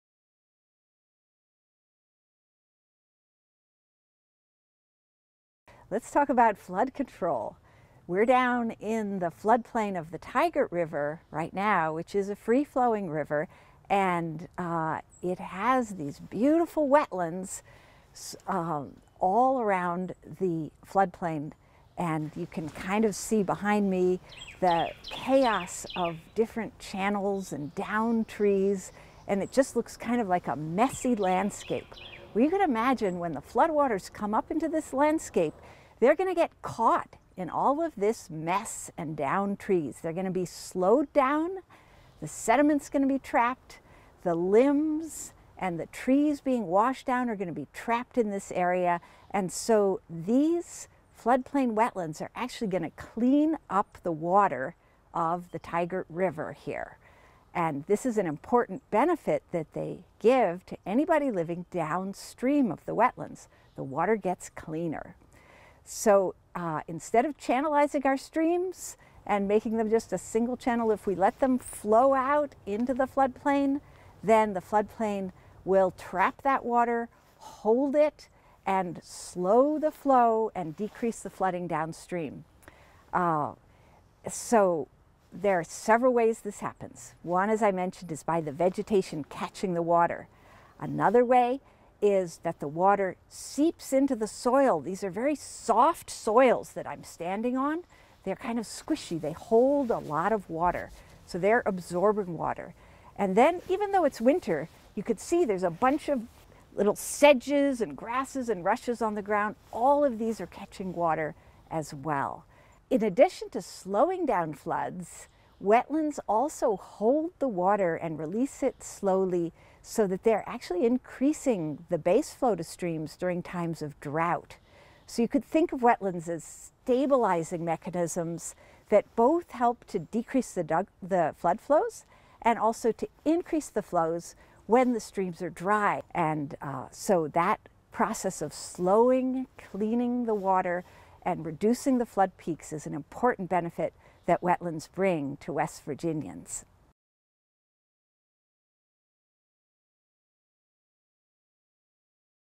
Wetlands and Flood Control
Wednesday, February 2nd is World Wetlands Day. All this week we'll be highlighting the environmental benefits of wetlands. Today - how wetlands help with flood control.